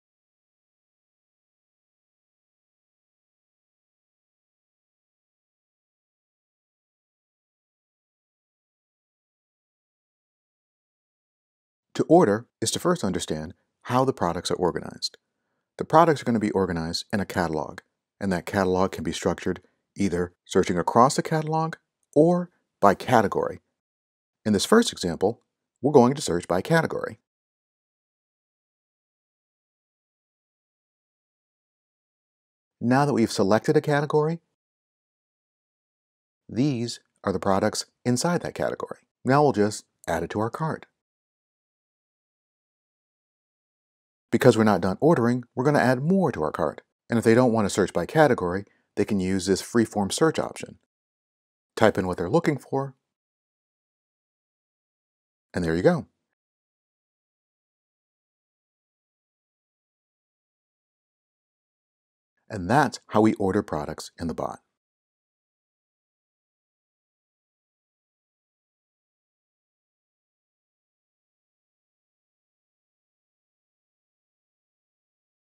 ManyChat Ordering Bot: Part One - Ordering in a Bot for Curbside Pickup
Demonstration of how a shopping cart ordering bot works in ManyChat using Integromat and MySql
There are three things people want in a simple ordering bot, the ability to move things in and out of a shopping cart, dynamic flows and the ability to pay for product fast. In this time of COVID-19 my team and I set out to build a messenger bot that would help small businesses. We can help them by giving them organized ordering for far less than anyone would imagine and the ability to advertise directly to a menu/shopping cart.
In this video we demonstrate how one would order products in the bot. The ordering uses categories and free-form search. The products or items returned in search are in a Facebook Messenger gallery that allows an add to cart mechanism.